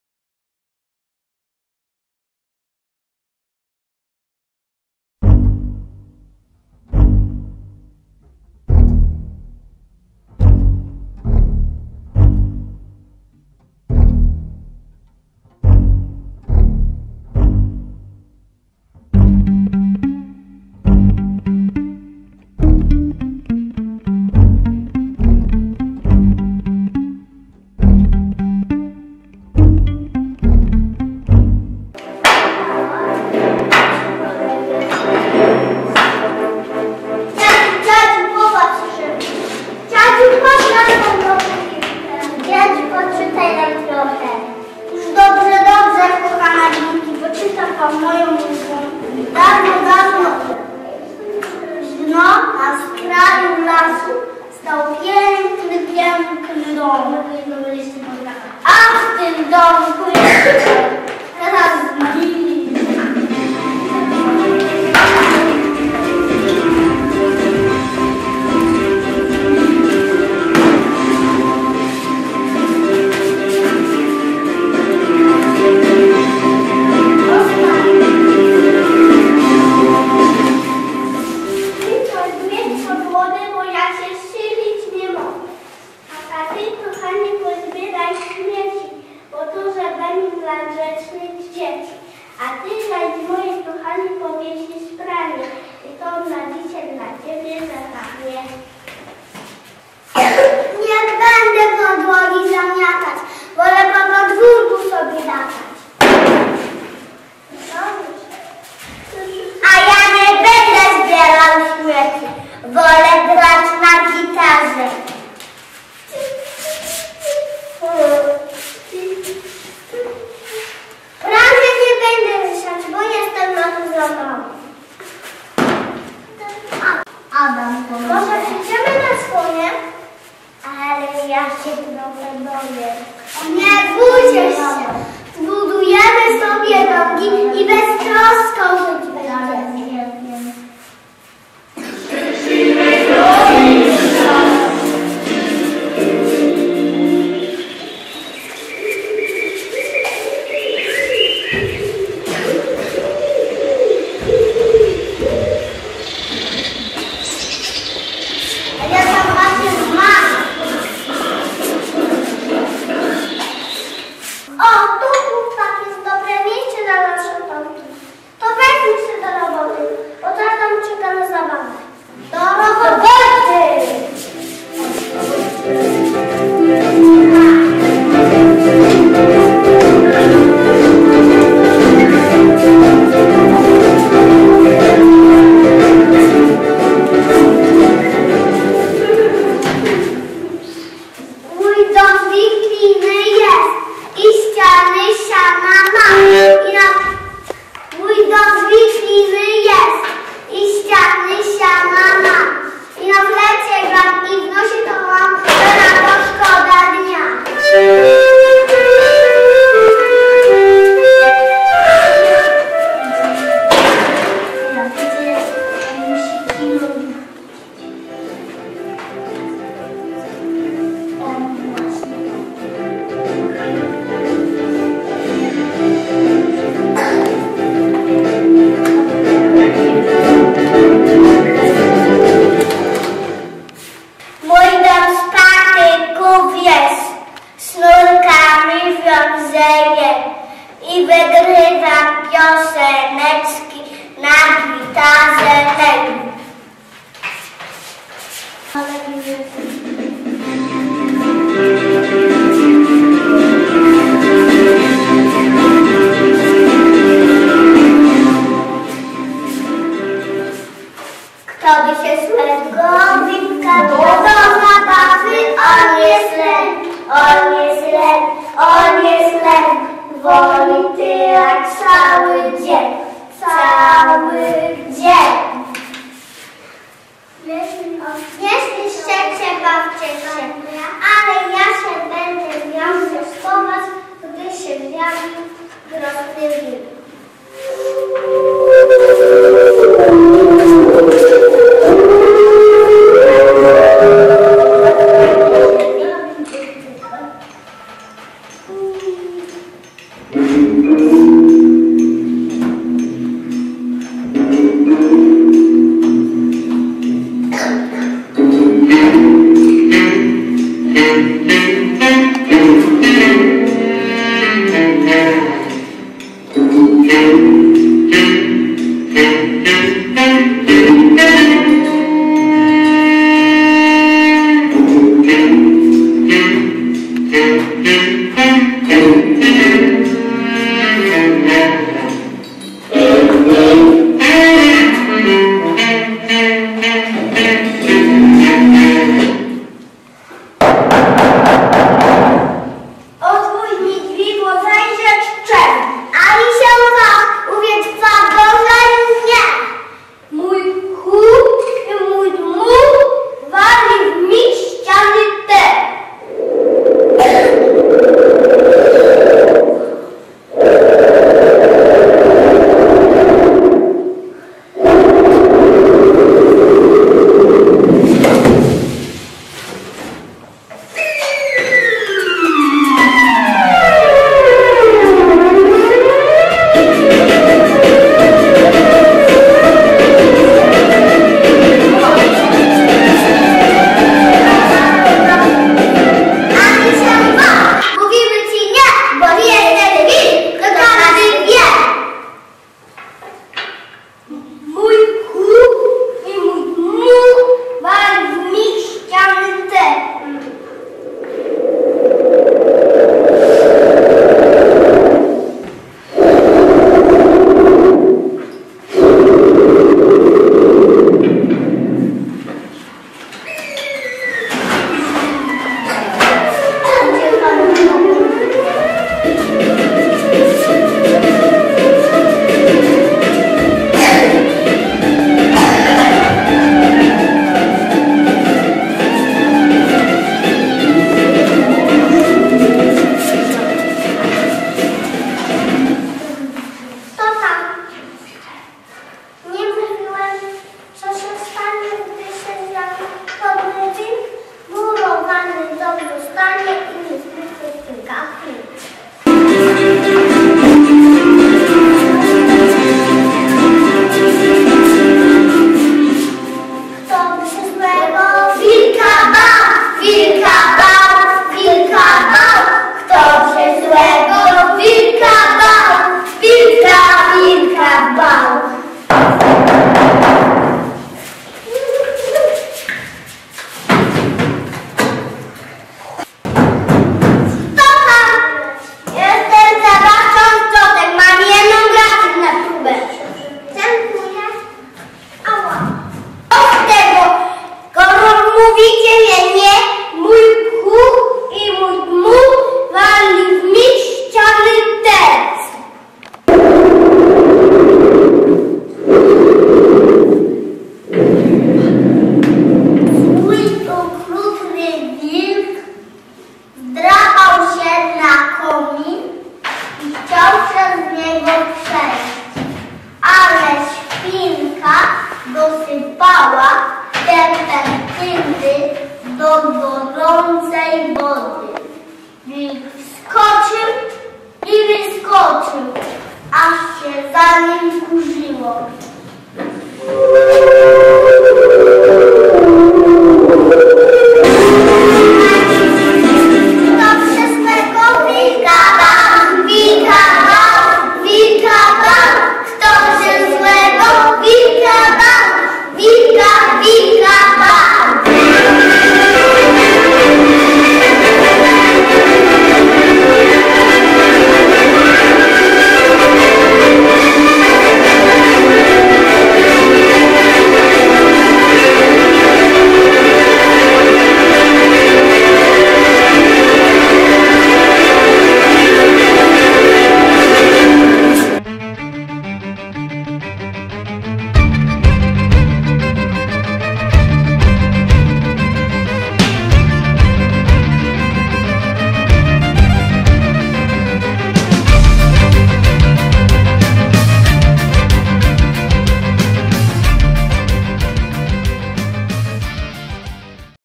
V SMYKOLANDIA 2015, "Bajka o trzech świnkach", Wieszczęta
ZSP w Wieszczętach przysłało na V Miedzynarodowy Multimedialny Przeglad Małych Form Parateatralnych "Smykolandia" 2015 program  pt. "Bajka o trzech świnkach".